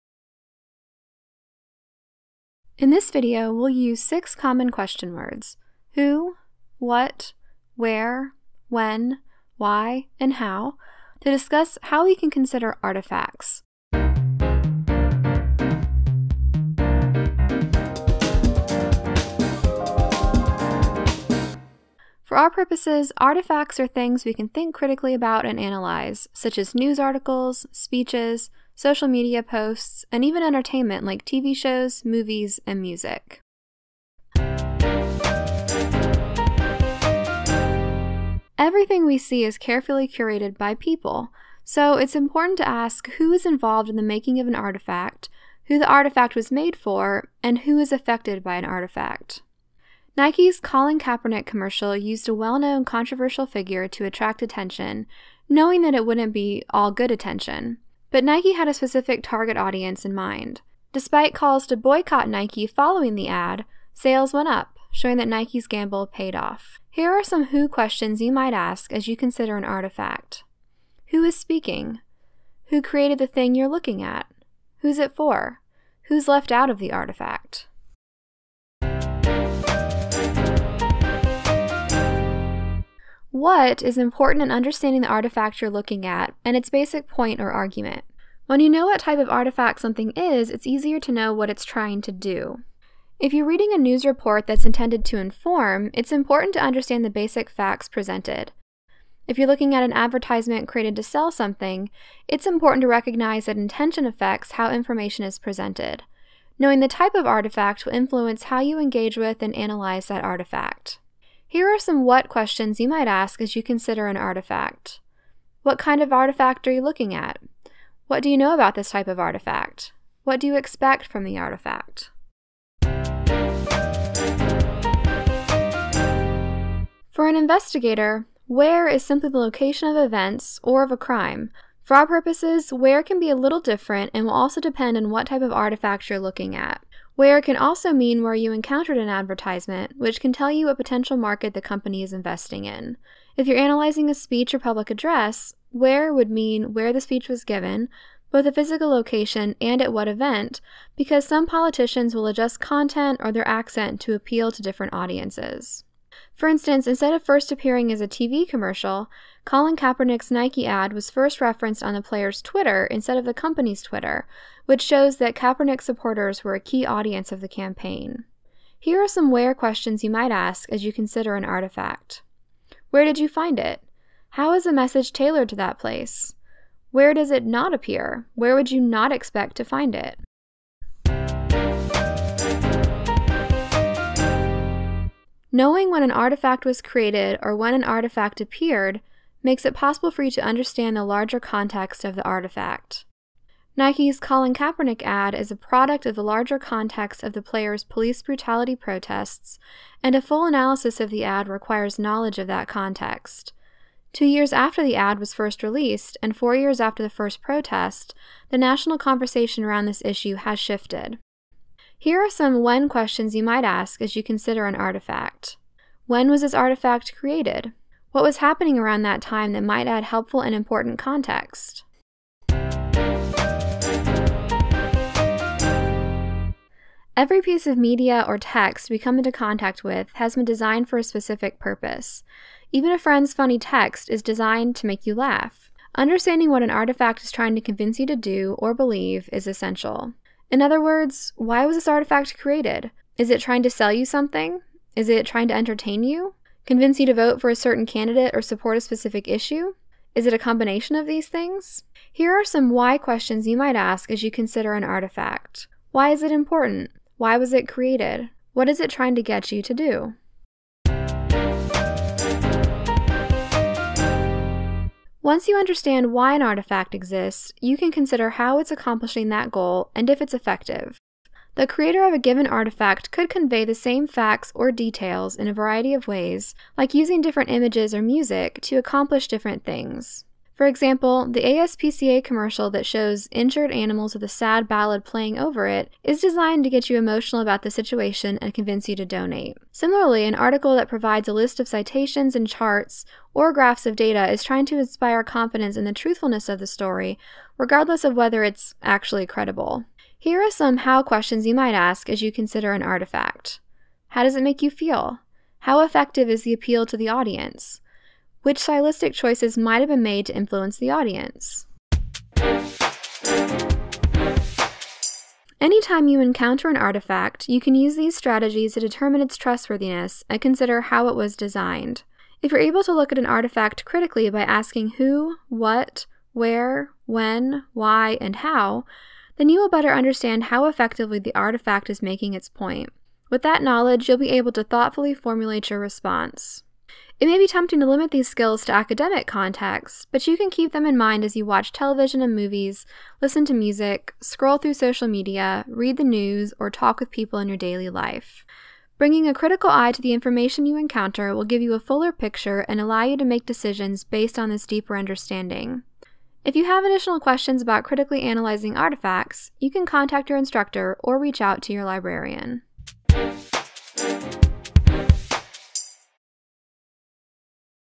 Applying the 5Ws
This video describes a tool - 6 question words - that can help you critically analyze artifacts like news articles, speeches, social media posts, TV shows, movies, and music.

Creators: Lisa Becksford, Katlyn Griffin, Kayla McNabb, Chloe Robertson
Contributors: Yemi Awotayo, Tim Becker, Julia Feerrar